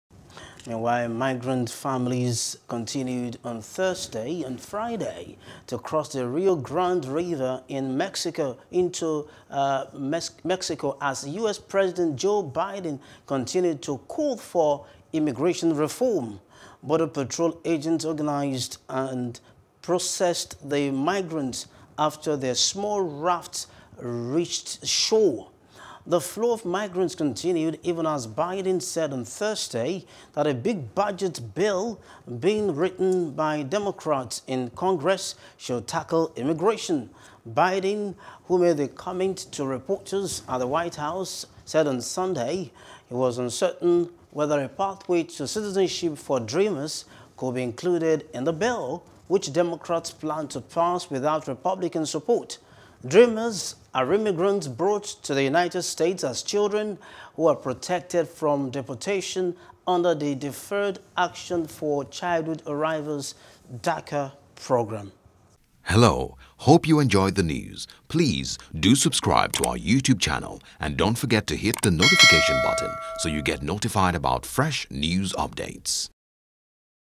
U.S. Immigration Reform: Migrants Continue To Cross Into Texas | FOREIGN
Migrant families continued on Thursday (July 29) and Friday (July 30) to cross the Rio Grande River into Mexico as U.S. President Joe Biden continued to call for immigration reform.